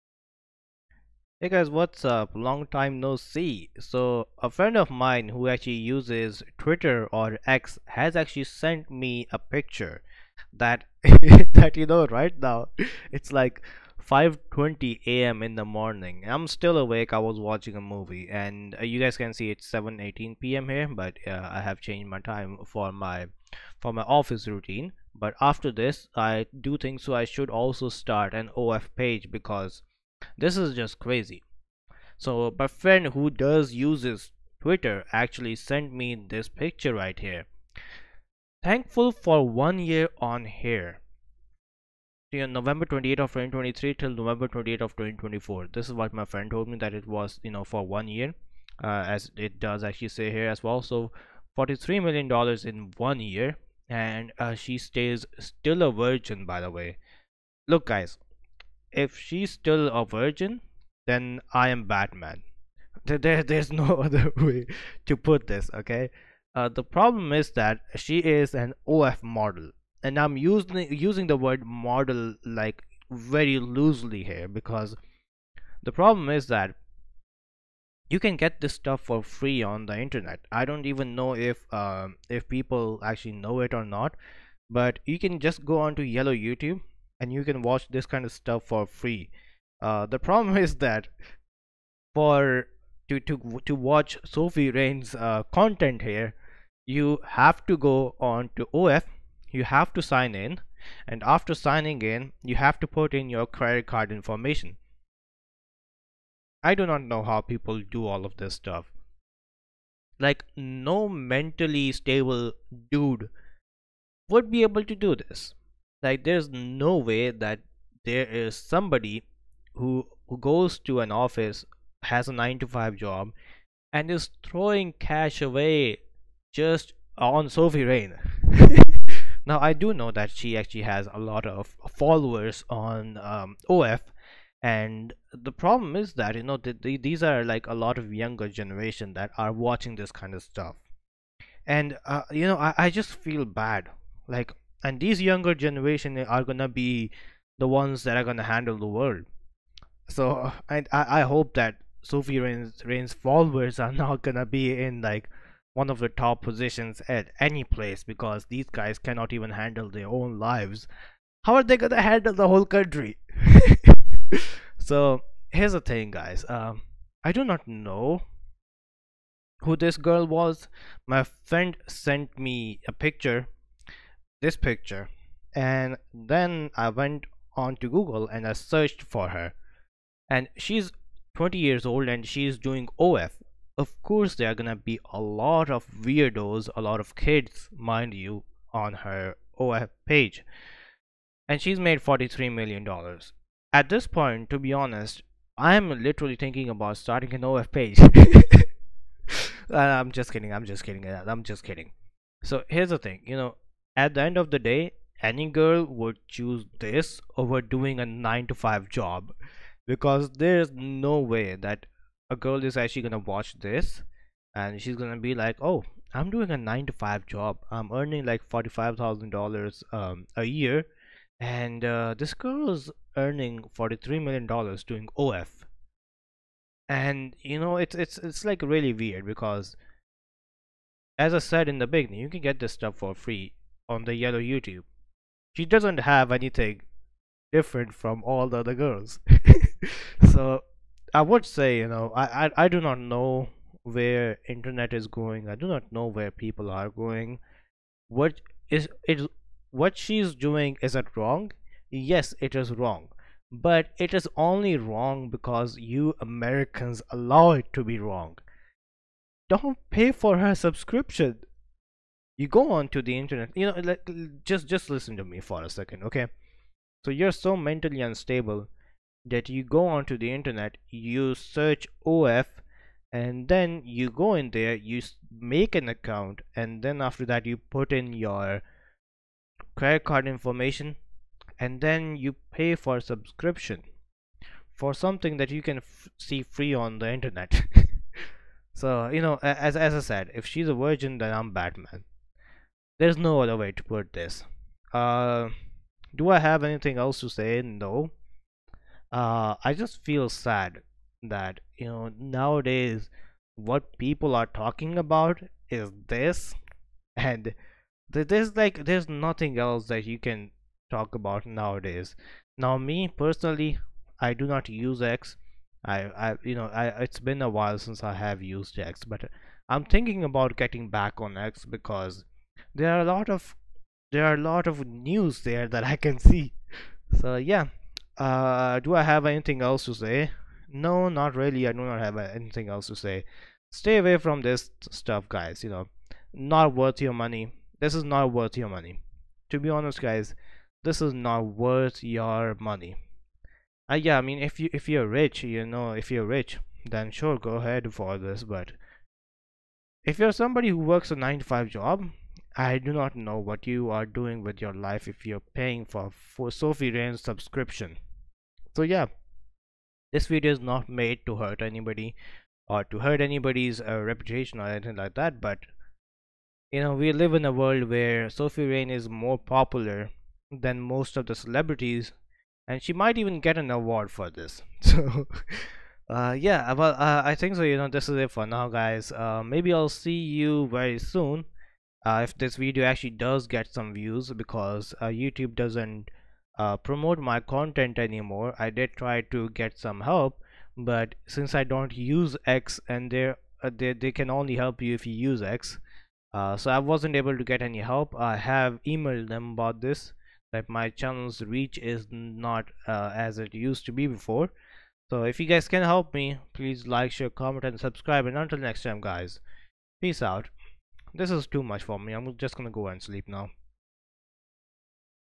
She's TRENDING Everywhere
Sophie Rain, a 20-year-old OF content creator, has garnered an impressive 43 Million in a year. She boasts 11 million OF subscribers, 5.2 million Instagram followers, and 2.5 million X followers. Rain's astounding earnings highlight the lucrative opportunities on OF for content creators.